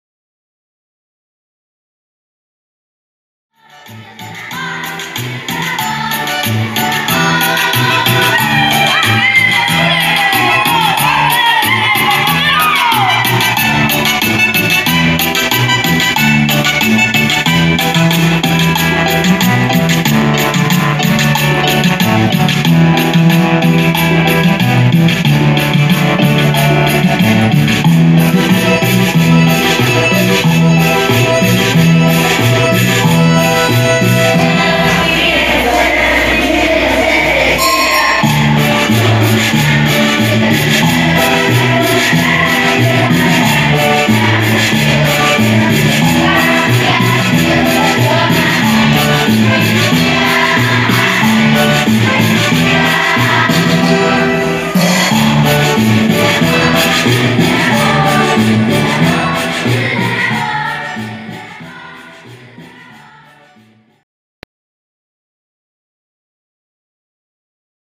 Alegría, diversión y entusiasmo. ‪#‎CCA6anochecolonial‬
Solo un poco de lo que se vivió en la noche colonial.